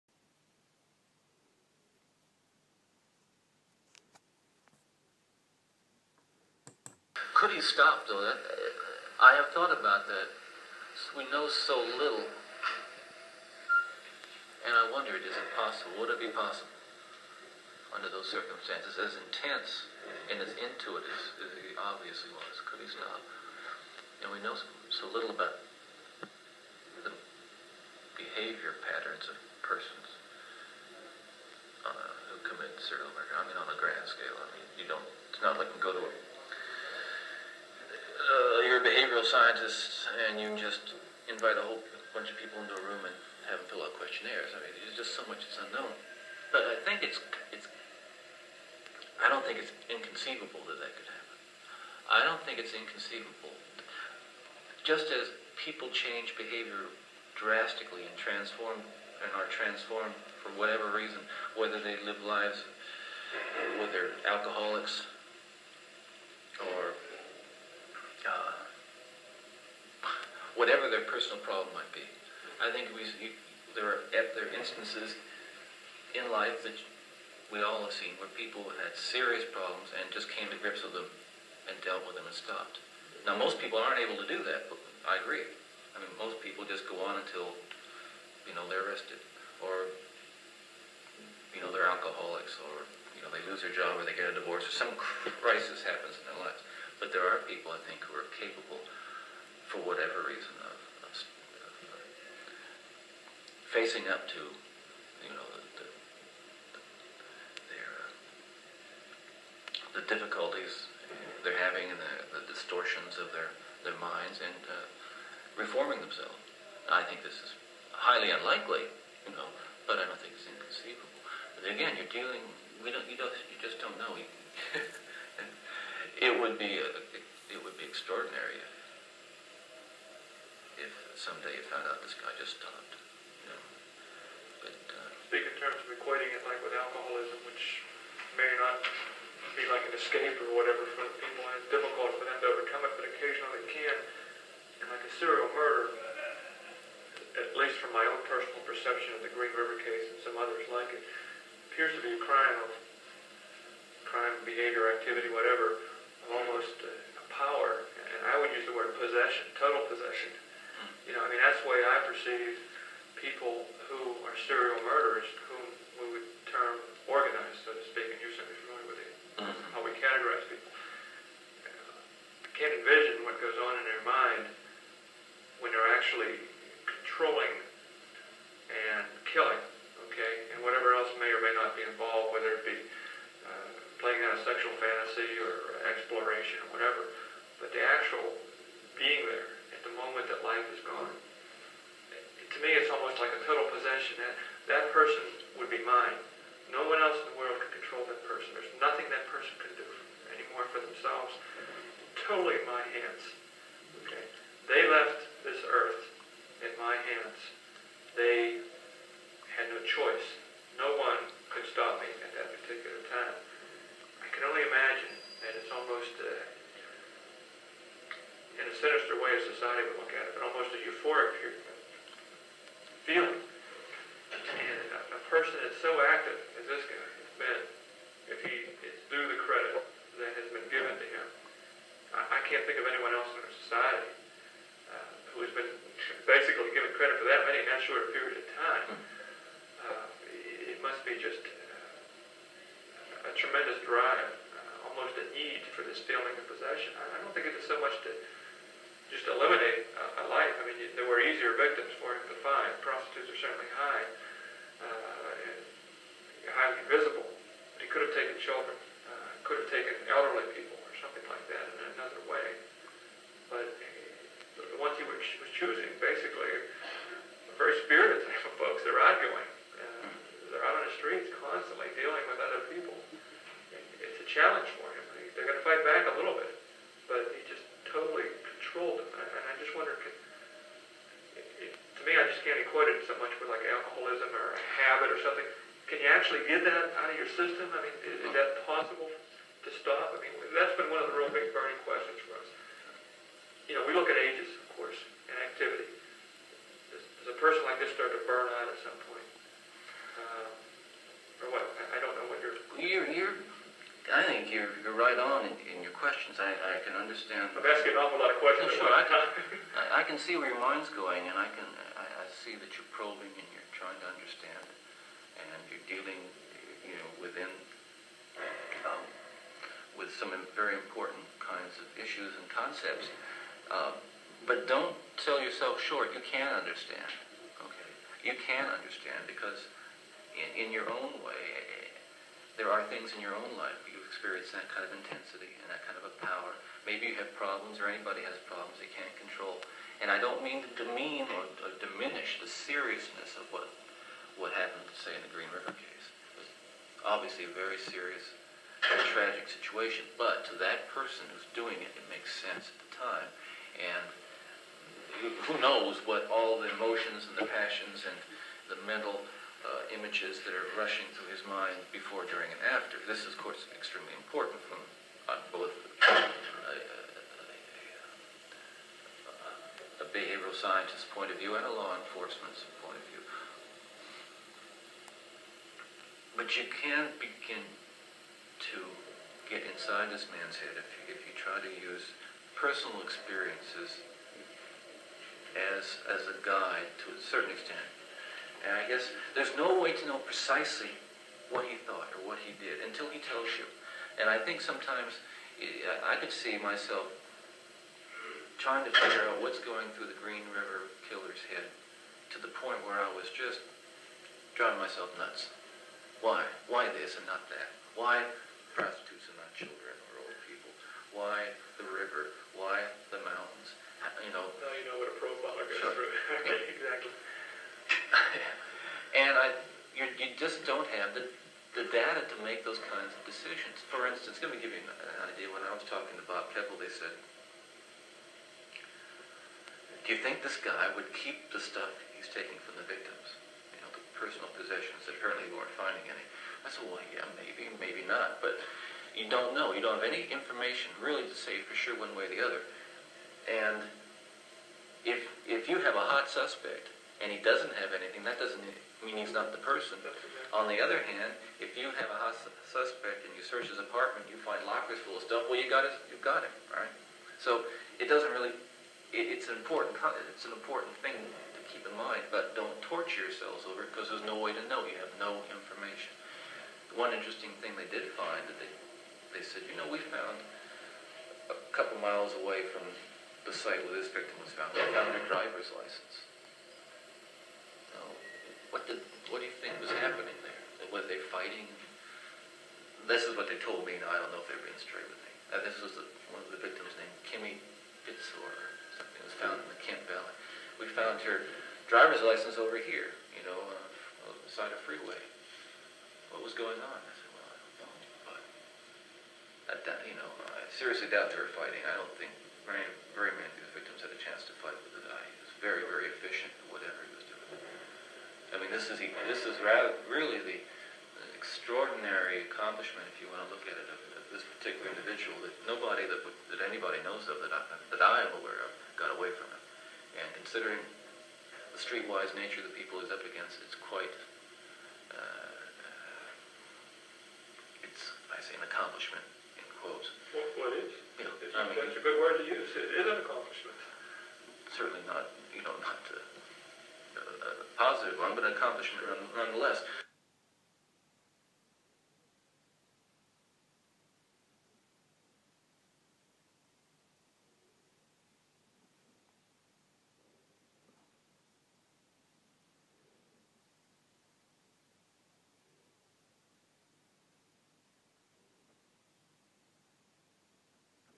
TED BUNDY WITH BILL HAGMAIER THEY LEFT THIS EARTH IN MY HANDS SEGMENT-3 PART-2 FEBRUARY 13TH 1986
bill and ted talking about grk gary leon ridgway. the riverman tapes